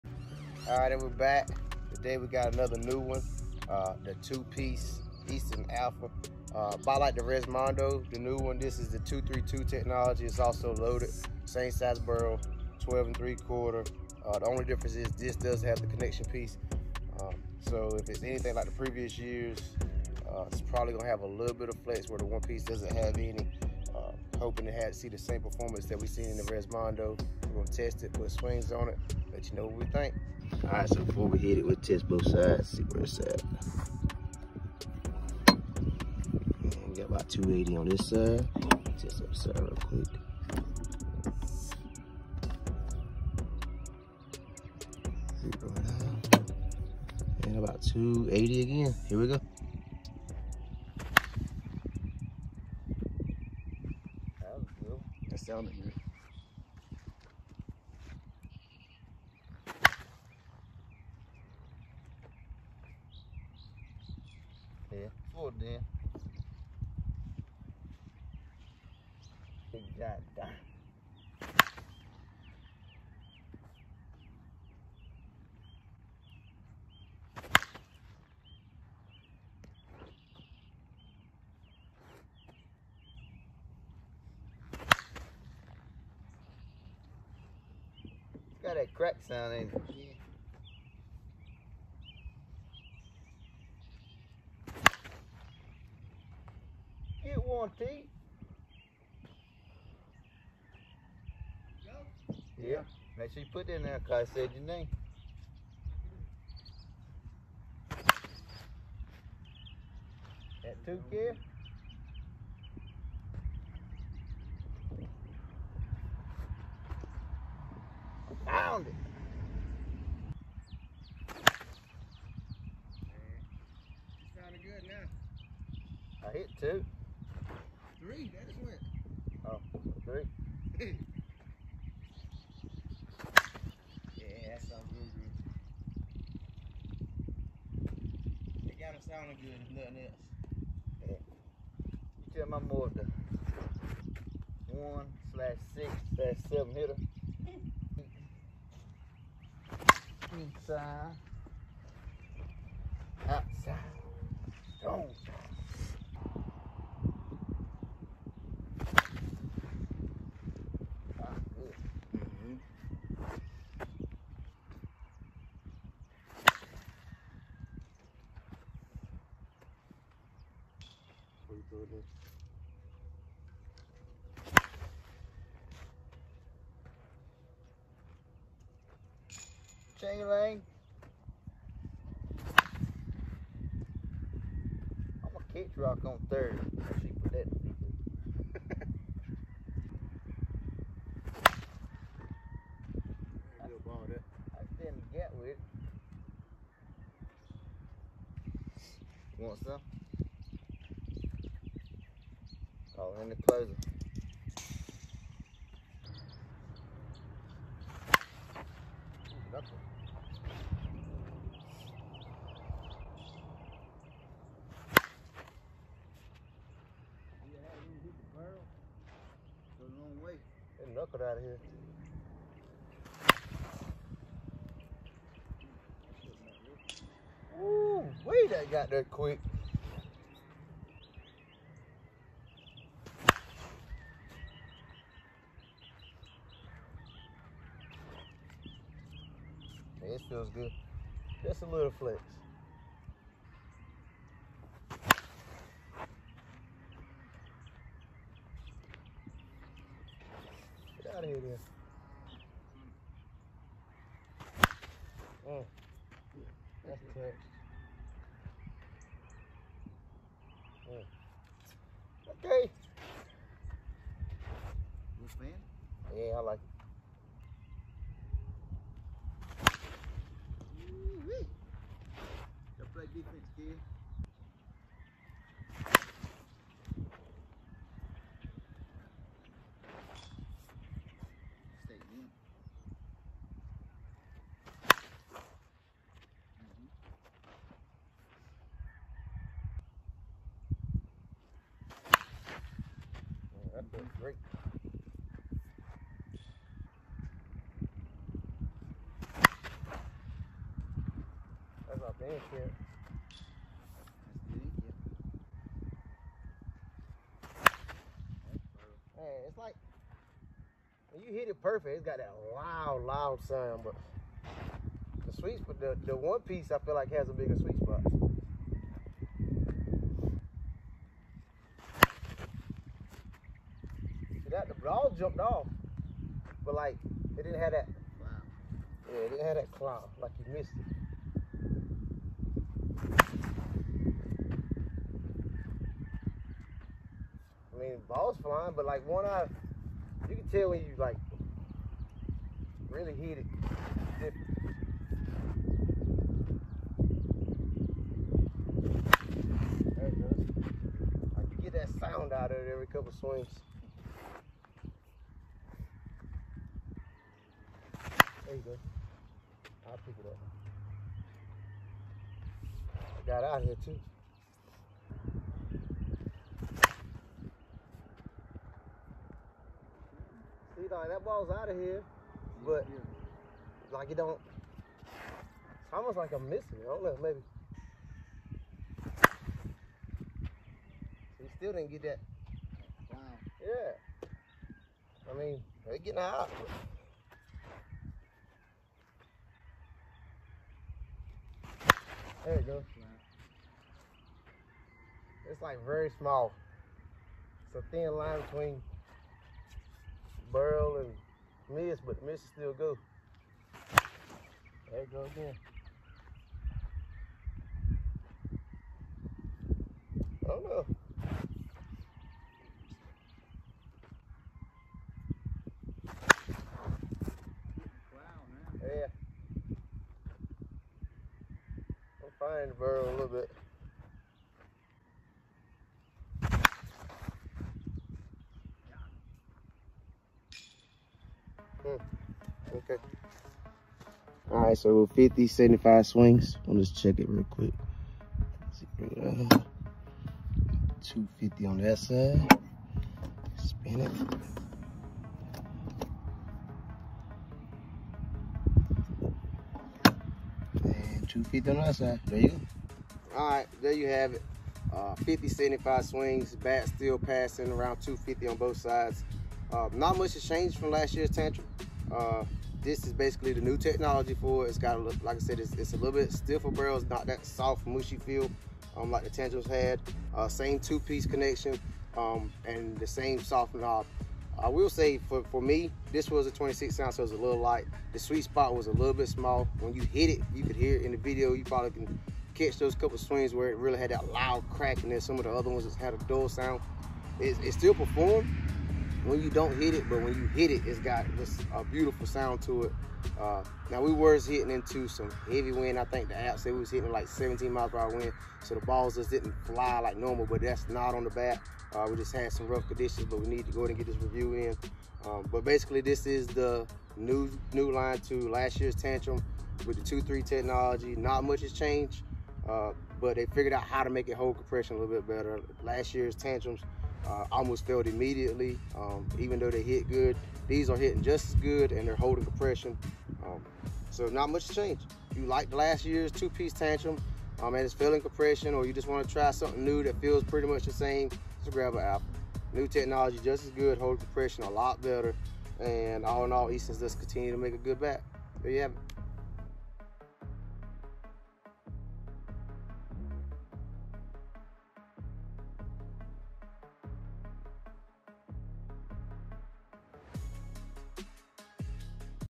Easton Alpha
New Easton Alpha, tantrum like performance with new 232 technology to hold compression much longer than last years lineup.